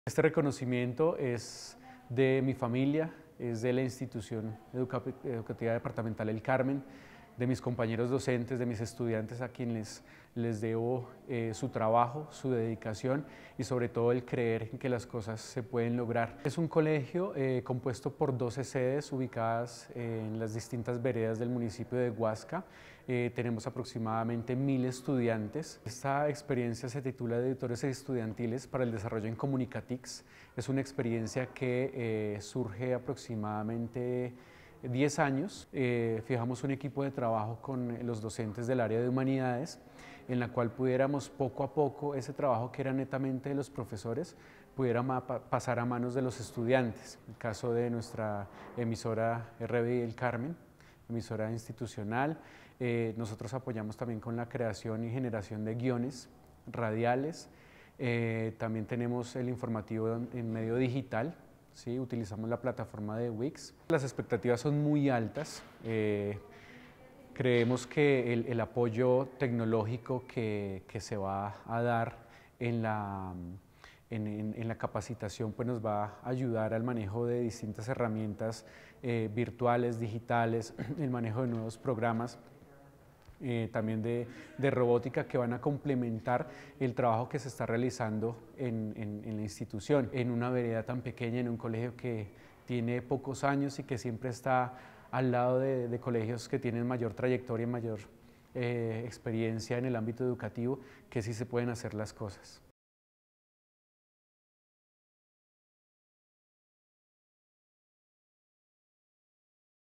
Jayson Camilo Malagón, becario ICT Training for Colombian Teachers Corea 2019
El docente Jayson Camilo Malagón, quien ganó una beca para vivir un proceso de inmersión en el sistema educativo de Corea del Sur, nos cuenta sobre su proyecto 'Editores estudiantiles para el desarrollo en Comunicatics'.